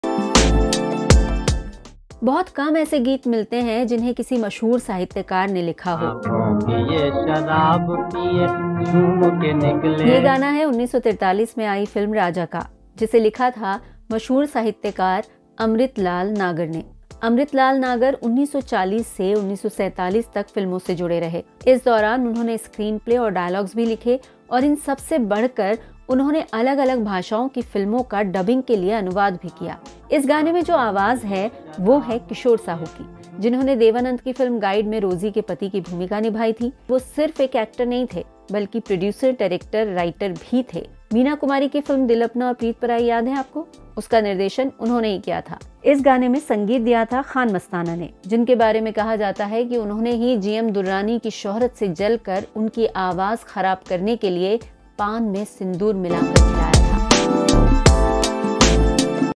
An Old Song Written By A Famous Writer And Many Famous Names Were Associated With It
एक पुराना गीत जिसे एक मशहूर साहित्यकार ने लिखा और बहुत से मशहूर नाम इससे जुड़े रहे

Kai sahitykaron ne filmon mein samay samay par kaam kiya lekin amuman sahitykaron ko cinema ki duniya zyada raas nahi aai. Aur phir waqt ke sath sath unke yogdan par dhool padti gai. ye bhi ek aisa hi gaana hai jise ek mashhoor sahitykaar ne likha par bahut kam log jaante hain ki unhone kabhi filmon ke liye bhi kuchh likha tha

You can check out these links If you are interested in Indian cinema